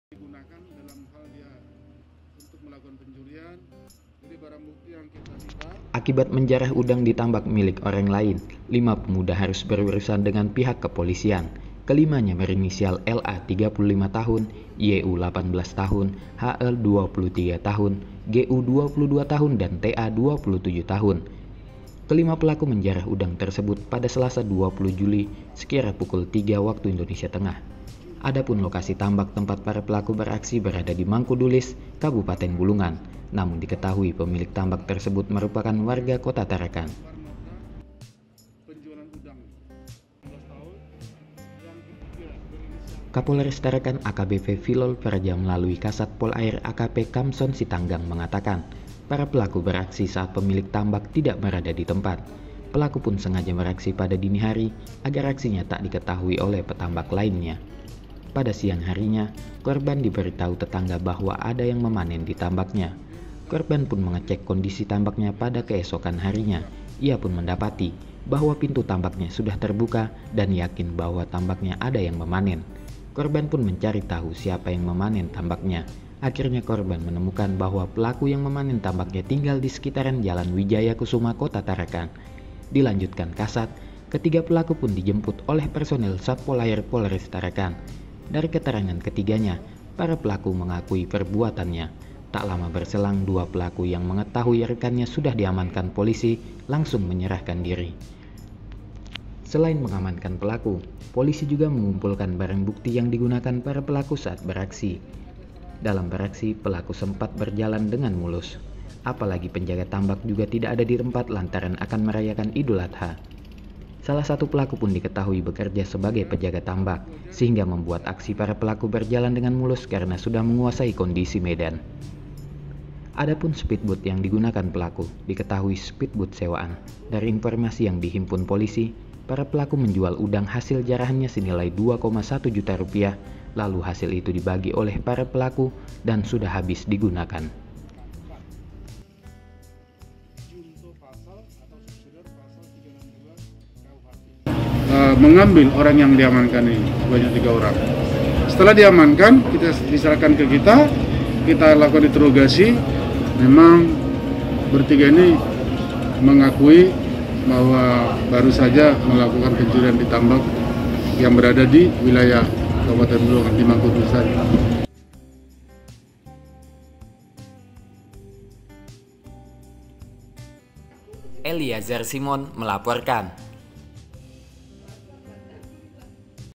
Menjarah Udang di Tambak, 5 Pemuda Berhasil Diamankan
Akibat menjarah udang di tambak milik orang lain, lima pemuda harus berurusan dengan pihak kepolisian. Kelimanya berinsial LA (35), YU (18), HE (23), GU (22) dan TA (27). Kelima pelaku menjarah udang tersebut pada Selasa 20 Juli sekitar pukul 03.00 WITA.  Adapun lokasi tambak tempat para pelaku beraksi berada di Mangkudulis, Kabupaten Bulungan. Namun diketahui, pemilik tambak tersebut merupakan warga Tarakan.
Reporter: Eliazar Simon